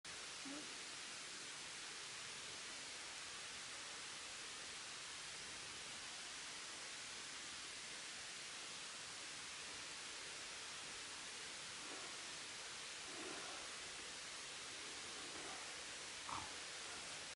Hair ice - a most unusual natural phenomenon
Also known as ice wool or frost beard, the conditions required for the formation of hair ice are extremely specific – a temperature slightly below 0 °C, moist and rotting wood, and the presence of a fungus called Exidiopsis effusa. In a complex process, the fungus causes a phenomenon known as ‘ice segregation’, which pushes water out of the wood pores, where it freezes and extends outwards into thin strands of ice.